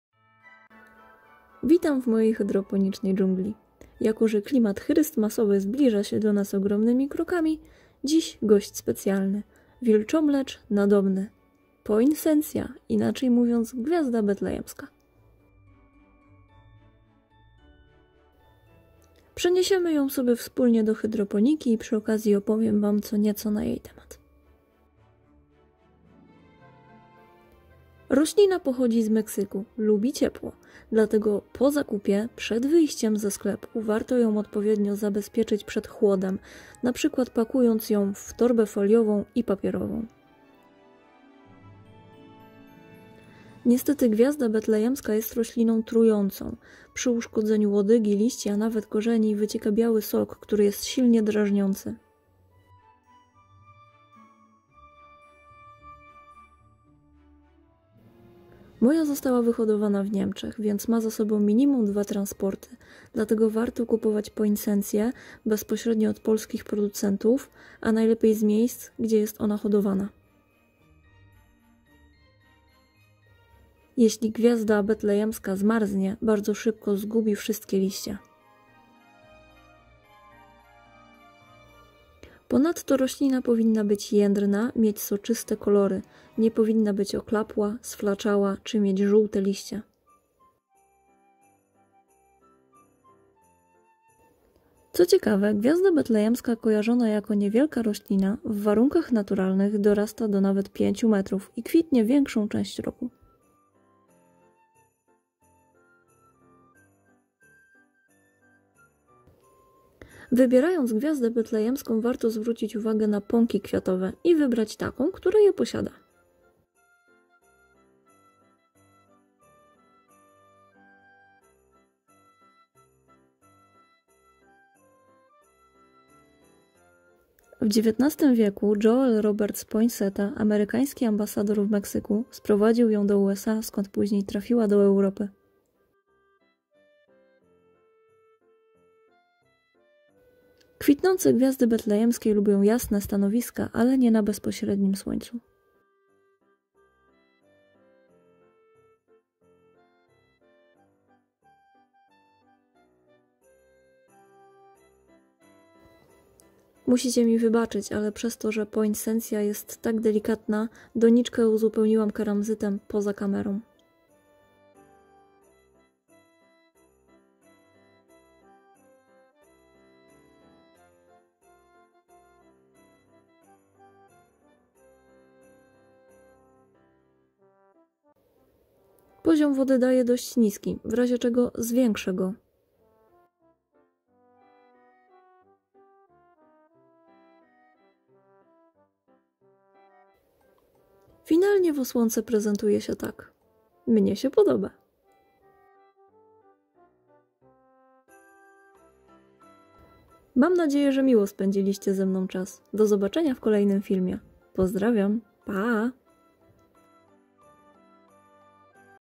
Gwiazda Betlejemska - Poinsecja - Wilczomlecz Nadobny . Z ziemi do hydroponiki
Witam serdecznie , Chrystmasowy klimat tuż tuż , czas na Gwiazdę :D .
Zakupione w aktualnej ofercie Lidla ( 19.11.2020 ) w cenie 3.99 za sztuke :)